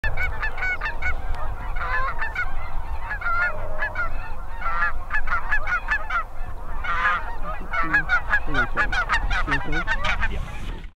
2 Geese Come into the Decoys PERFECTLY!! 😍👌🏼
These two geese absolutely do it perfectly right in the decoys. I love me some mallards but these big geese coasting into the decoys really close to the ground is really awesome too! I promise that they didn't make it out of the decoy spread!